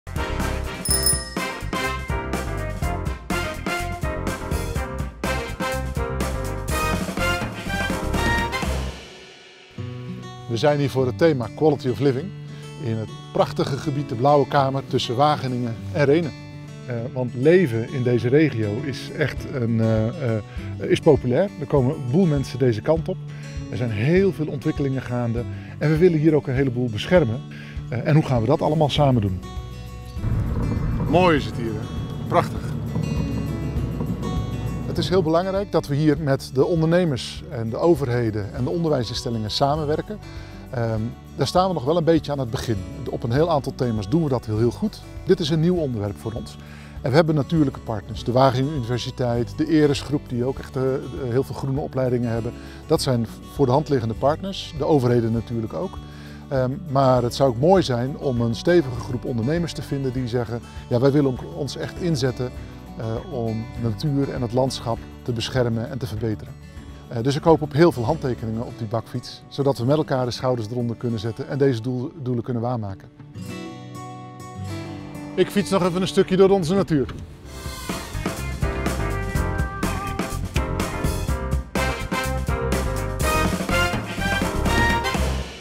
Fiets met ons mee door de regio - deze week: Arjen Droog
Fiets met ons mee door de regio. Deze week fietst Arjen Droog, secretaris-directeur van Regio Foodvalley, door de regio. Hij tekent de Strategische Agenda voor het thema Quality of Living (onderdeel natuur en landschap).